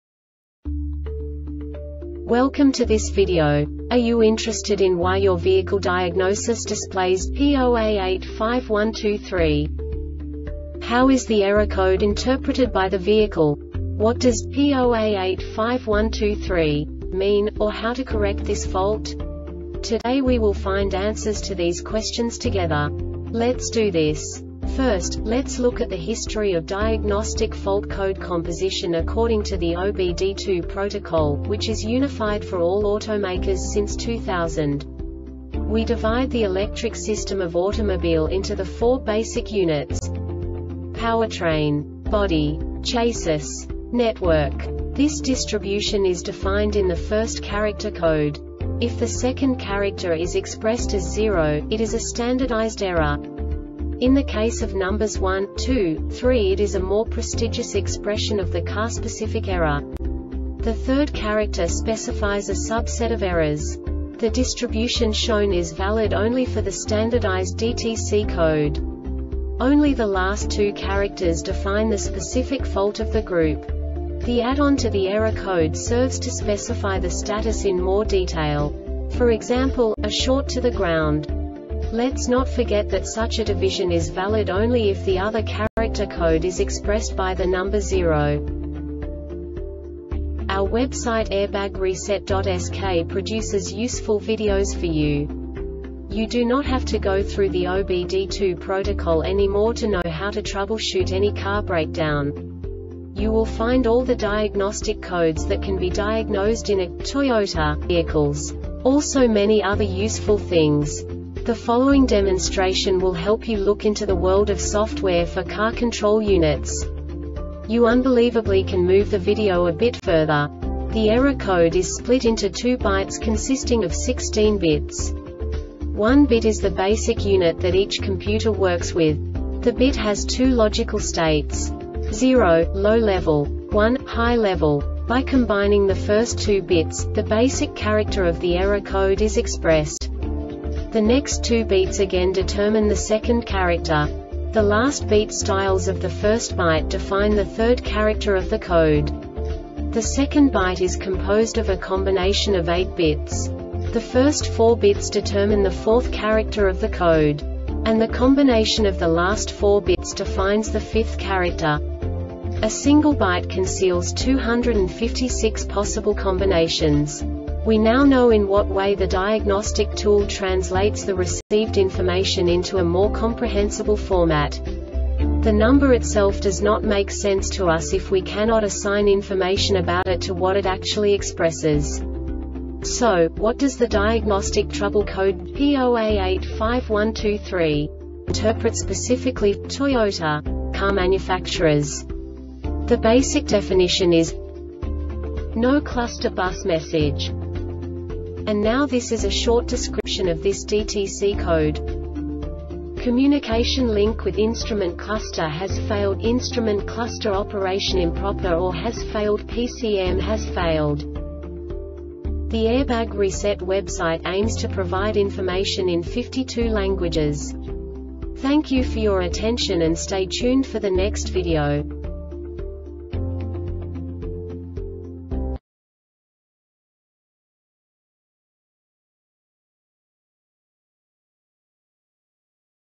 DTC Toyota P0A85-123 Short Explanation
Contents:
0:21 Basic DTC analysis according to OBD2 protocol standard.
1:48 Insight into programming
2:58 A diagnostically understandable description of the Diagnostic Trouble Code
3:05 Basic error definition

Make:
Toyota
Code:
P0A85 123
Definition:
No Cluster Bus Message
Cause: